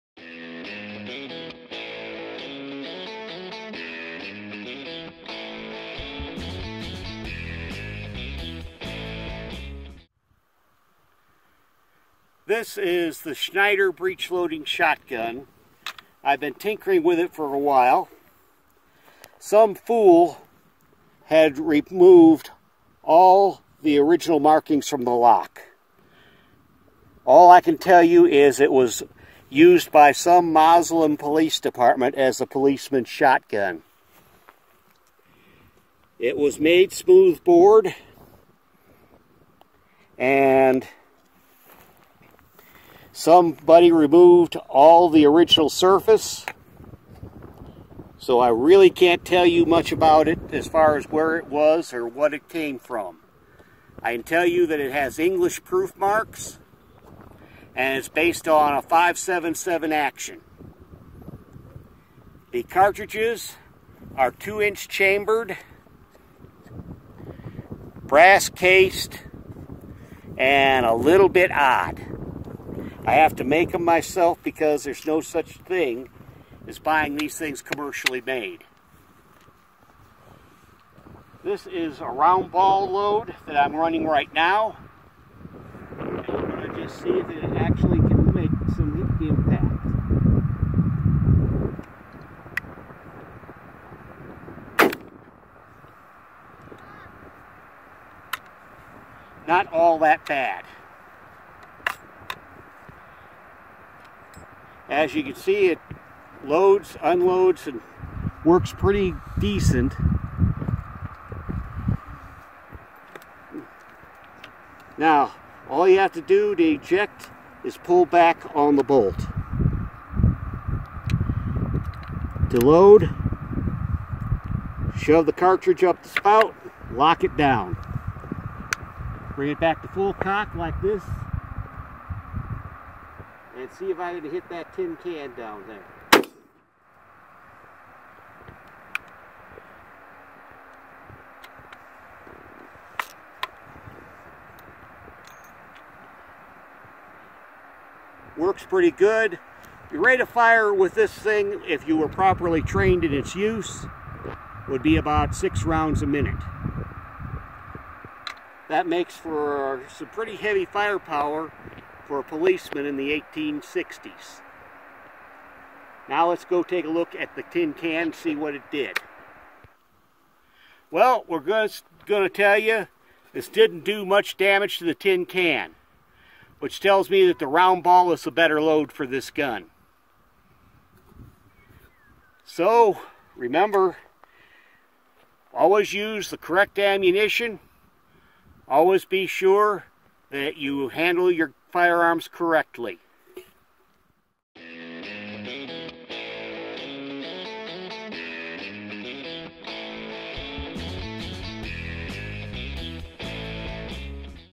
Old Breach loading Shotgun test fired and reviewed. Lets hope it goes well.
In this video we take the Snyder Breach loading shotgun that we talked about in a previous video and test it out. Watch the video and see how well this equipment performs.